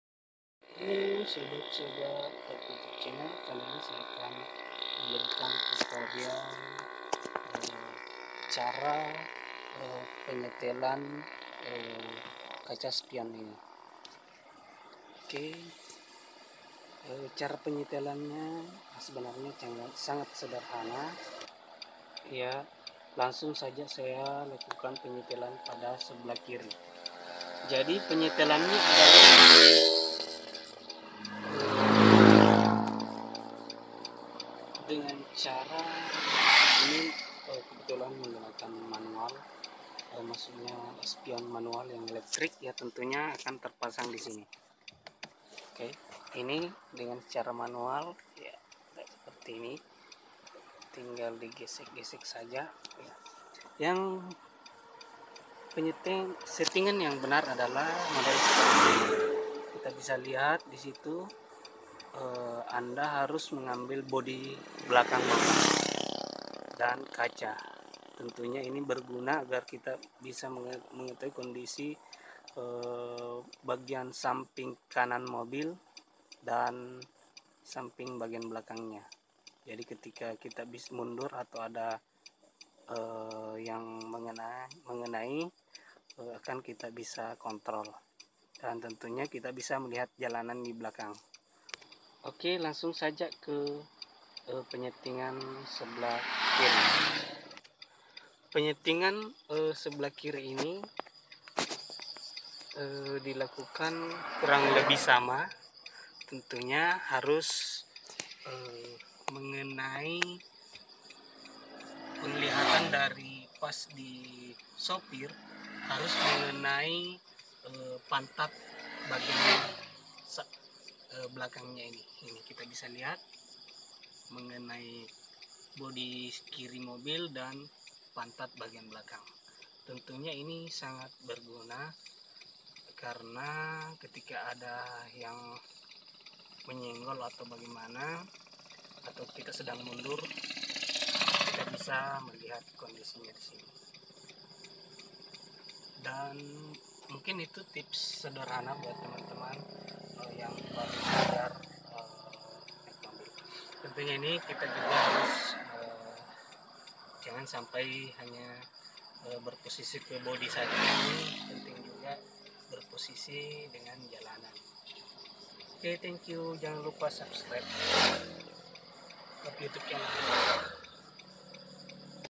CARA MENYETEL KACA SPION MOBIL DENGAN BENAR
Tentu jangan sepelehkan penyetelan kaca spion mobil anda karena pengemudi yang bergantian yaitu tinggi badan pengemudi tentunya akan berpengaruh penyetelah kaca spion ini.